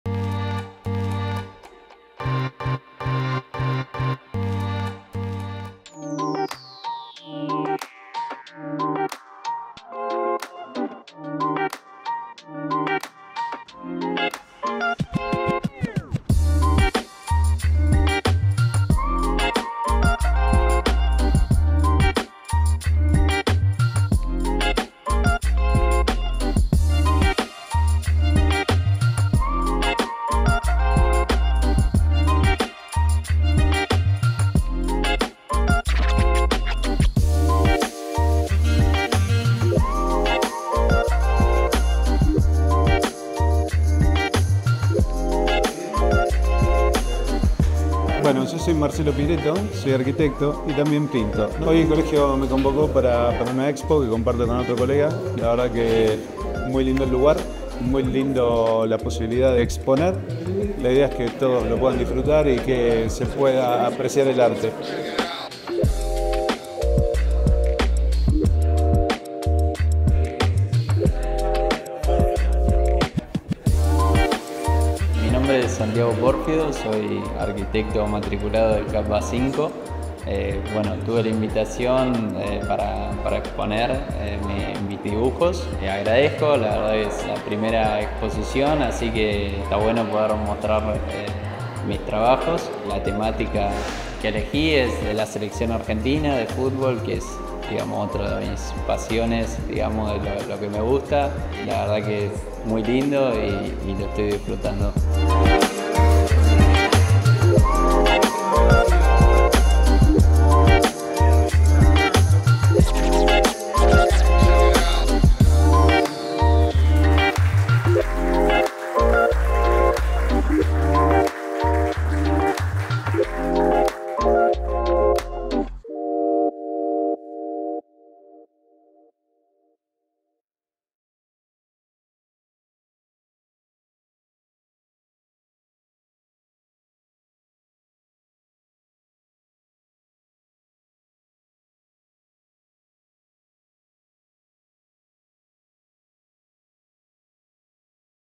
2024.05.10 - Muestra de Arte (Pieretto-Pórfido)
SE INAUGURÓ UNA NUEVA MUESTRA EN SEDE DISTRITAL

El pasado viernes 10 de mayo, se inauguró una muestra en la Sede Distrital del Colegio de Arquitectos - Distrito 5. En esta oportunidad exponen los arquitectos y artistas Santiago Pórfido con su muestra titulada” Muestra de Selección” y Marcelo Pieretto presentando su muestra “¡La Magia del esfuerzo!”